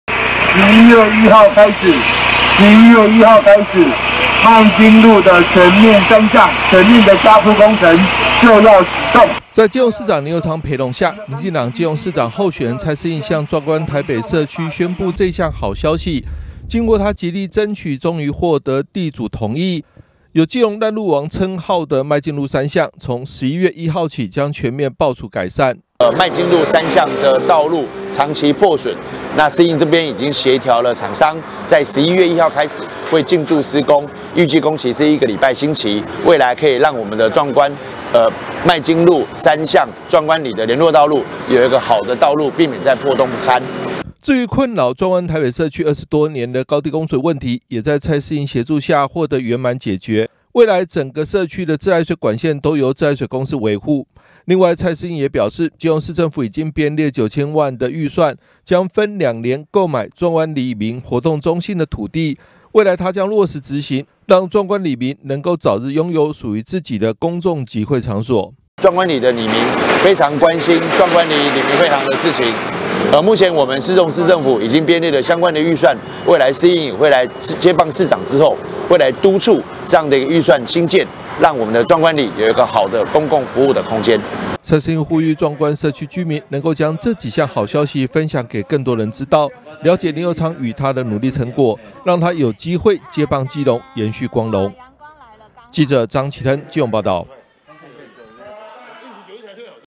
交通 基隆|爛路 坑洞|刨鋪 道路|社區 聯外道|蔡適應 林右昌|高地 供水|2022 中嘉新聞|擺脫爛路王汙名 蔡適應爭取 麥金路3巷11月1日起道路刨除重鋪
基隆爛路王麥金路3巷有救了 蔡適應說服地主同意11月刨鋪改善

（中嘉吉隆新聞記者張啓騰/基隆報導）因為道路品質差而有基隆爛路王稱號的麥金路3巷終於有改善機會，民進黨基隆市長候選人蔡適應宣布在獲得地主同意後，11/1起麥金路3巷將全面刨除重鋪，這條壯觀台北社區通往國道一號的替代道路未來將不會再坑坑洞洞，另外基隆市政府也已經編列九千萬的預算分兩年購買土地建造壯觀里民活動中心，蔡適應表示未來接棒後將落實執行，讓壯觀里民能早日擁有屬於自己的集會場所。